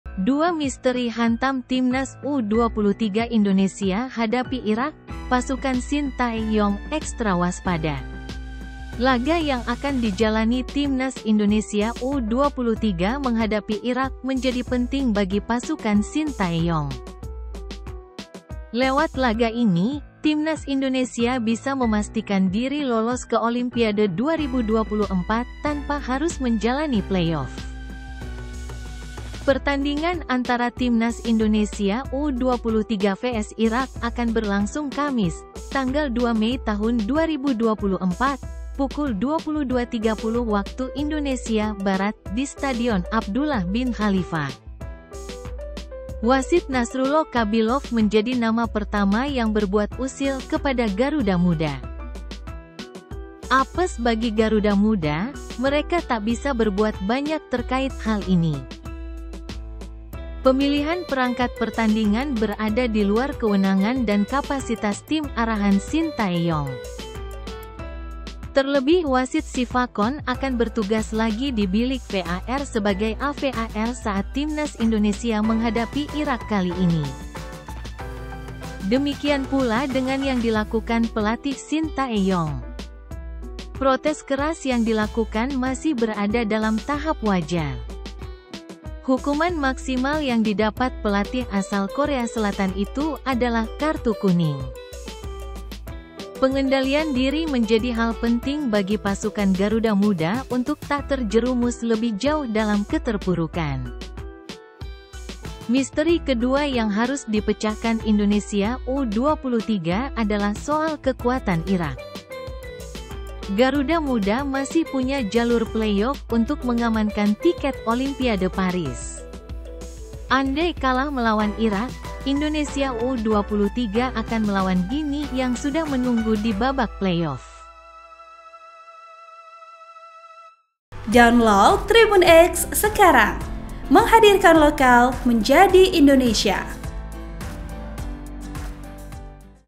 Laga akan dijalani Timnas Indonesia U23 menghadapi Irak menjadi penting bagi pasukan Shin Tae-yong.
TRIBUNNEWS.COM - Lewat laga ini, Timnas Indonesia bisa memastikan diri lolos ke Olimpiade 2024 tanpa harus menjalani playoff.

Pertandingan antara Timnas Indonesia U23 vs Irak akan berlangsung Kamis (2/5/2024) pukul 22.30 WIB di Stadion Abdullah bin Khalifa.

Wasit Nasrullo Kabilov menjadi nama pertama yang berbuat usil kepada Garuda Muda.

VP: PaceBatak